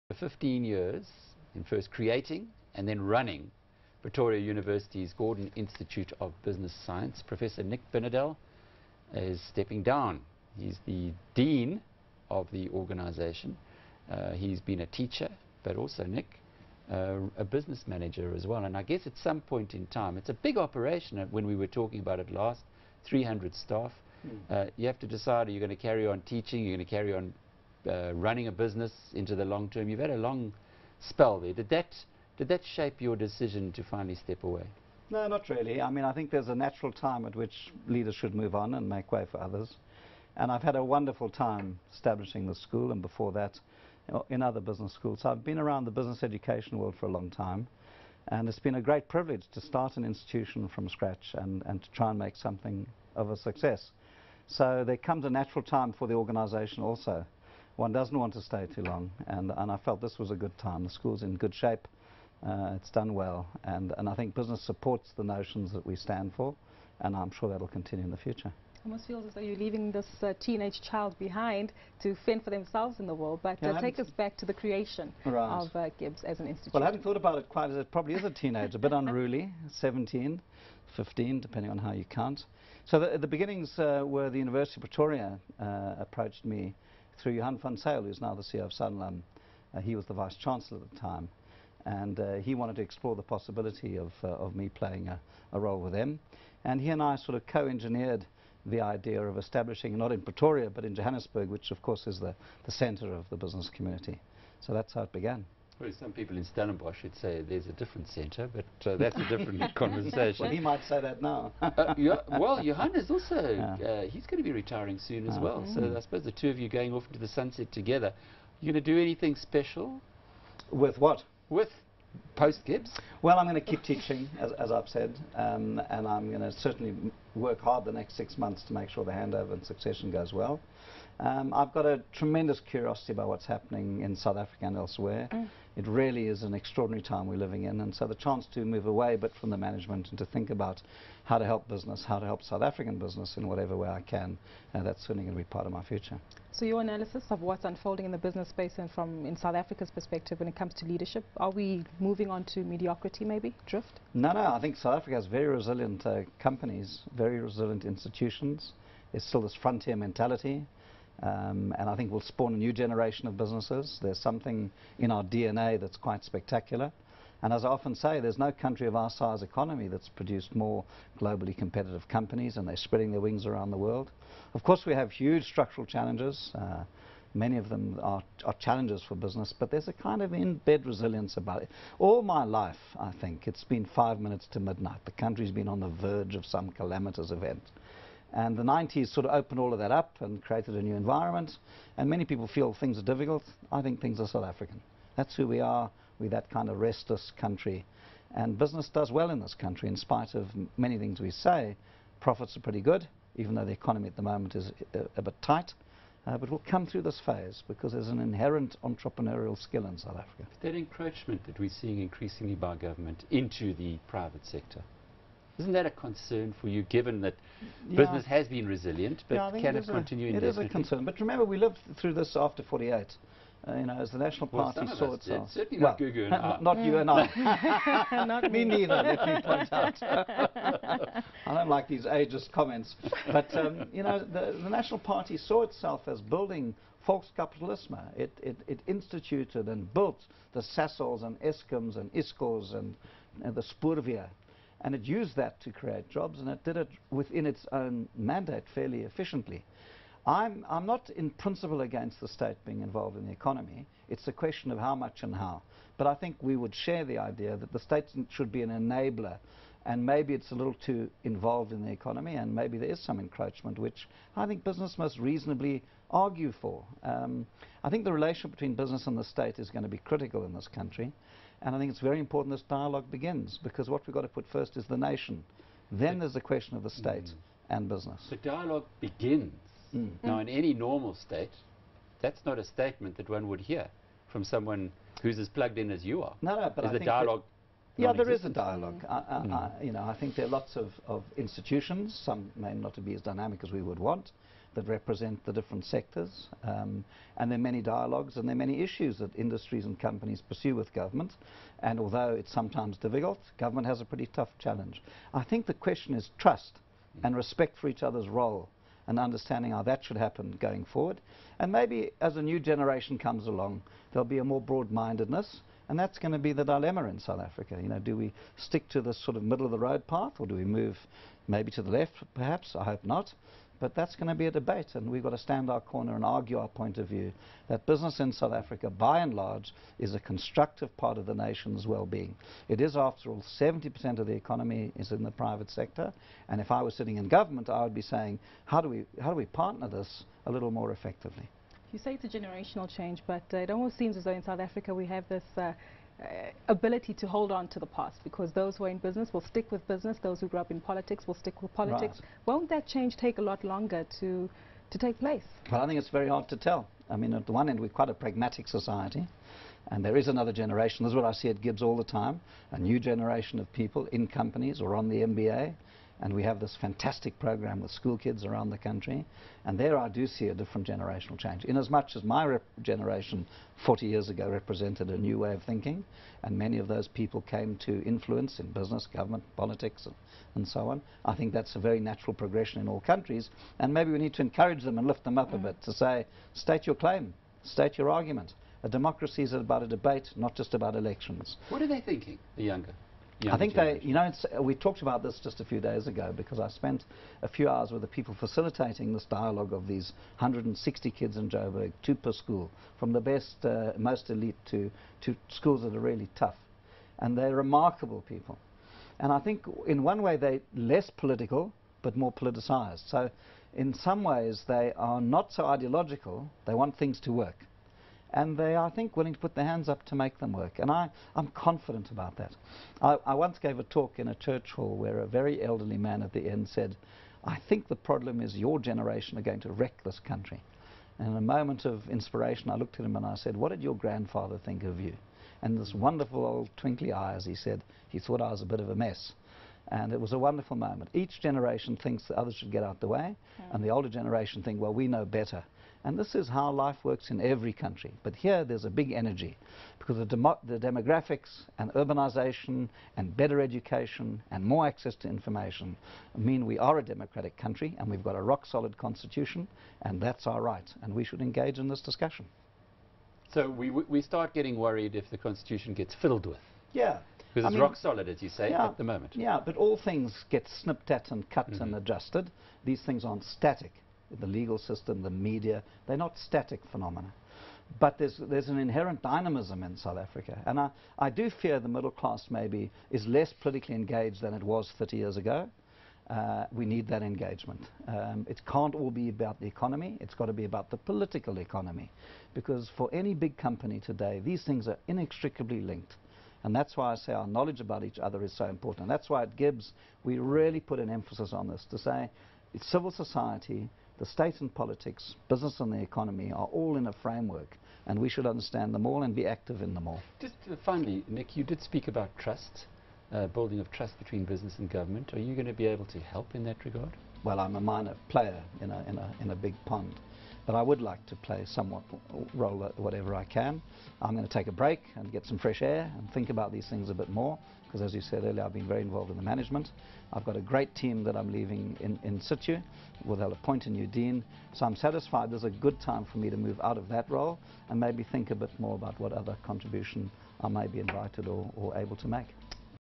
Prof Nick Binedell steps down as dean of GIBS
Original Publish Date: || Tue, 12 Aug 2014 09:13:12 GMT || After 15 years of heading Pretoria University's Gordon Institute of Business Science, Professor Nick Binedell is stepping down as the Dean of the institute.  He joins CNBC Africa for more.